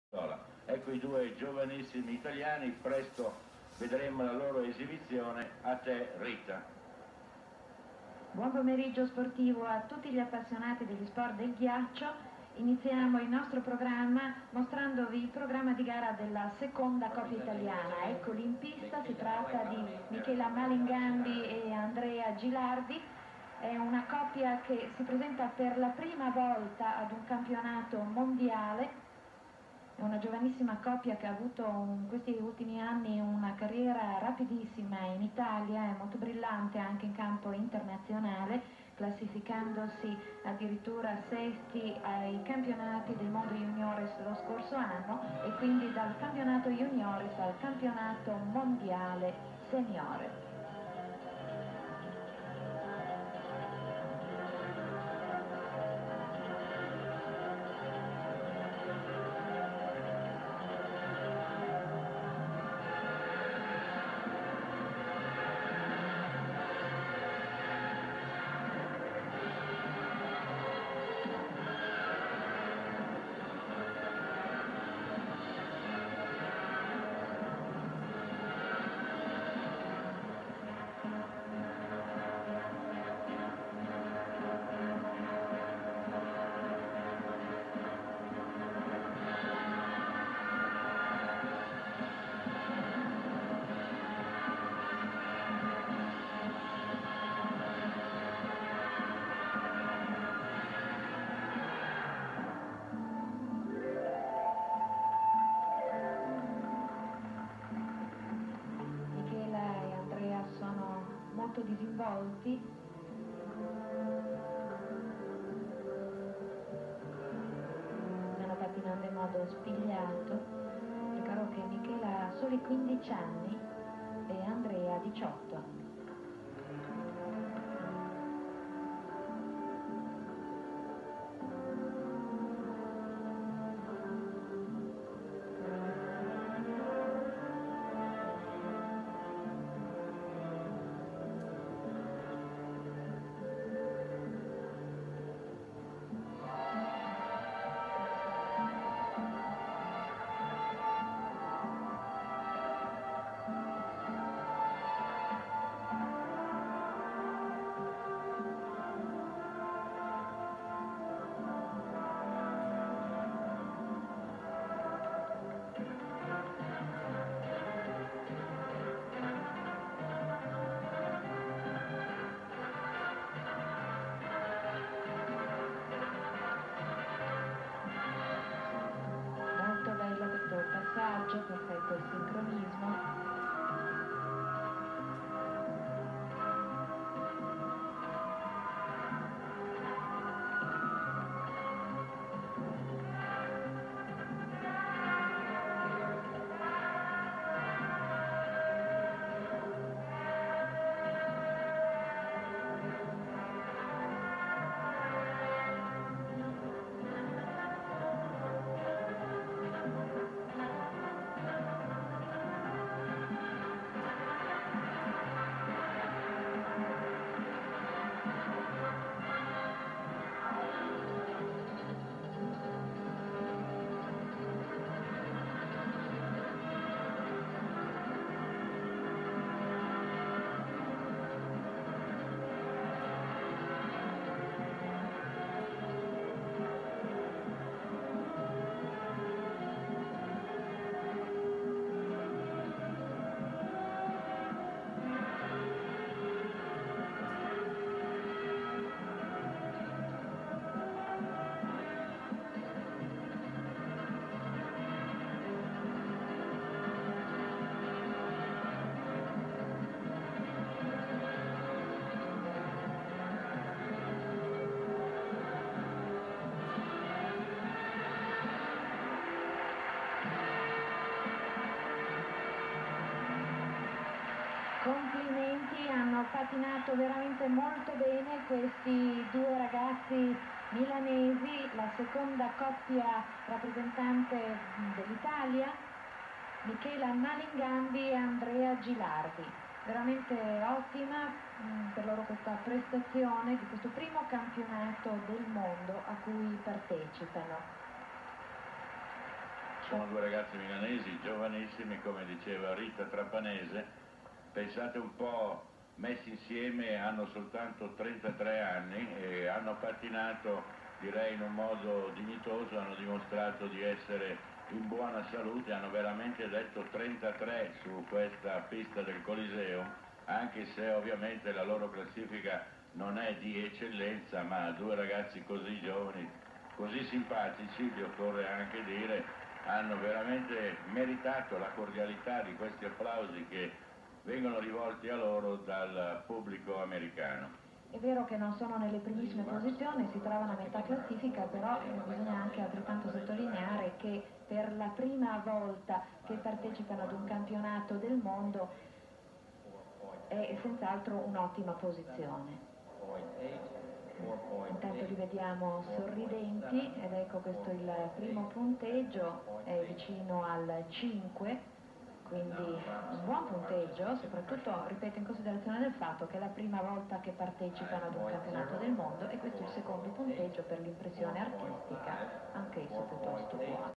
Michela Malingambi - Andrea Gilardi Free Dance 1987 Latin Medley
World Figure Skating Championships 1987 Cincinnati USA